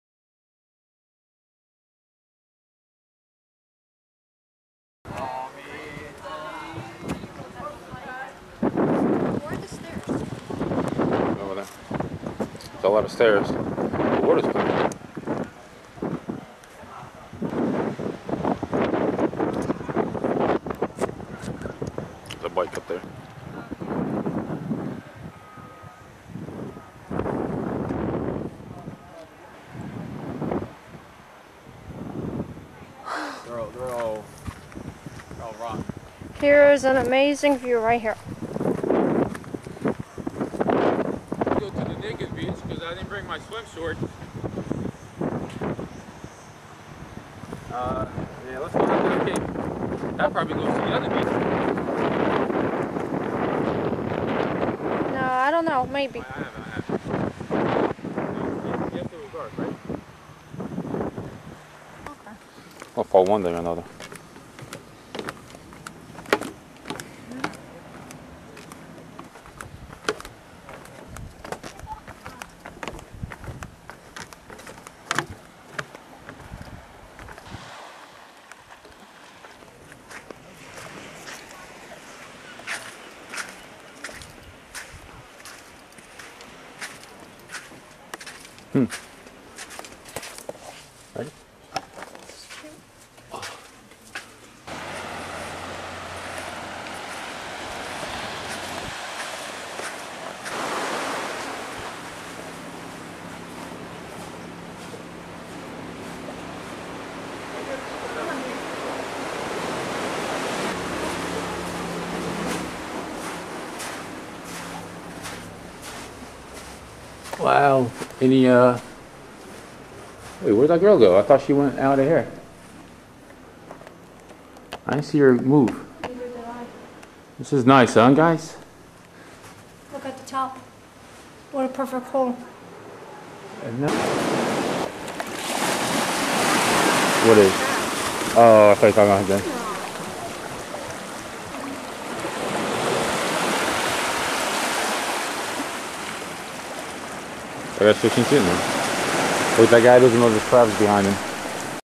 Portugal, Algarve Lagos
On our honey moon trip, we stopped at the city of Lagos in Algarve Portugal.  everything was awesome looking and too bad we only had 1 day there... the beaches were marvelous, and the rock formation along the south coast gives it an awesome setting.  many caves are formed from the crashing water, and some can be visited by boat... it's like something from the Goonie's movie and you wonder if any are large enough to house a treasure.
this is one of the beaches we stopped briefly... the water was calm and clear, and just nice to relax.
as always, Portugal has a topless policy if you would like to be topless, so for tourists this is always a plus right? :)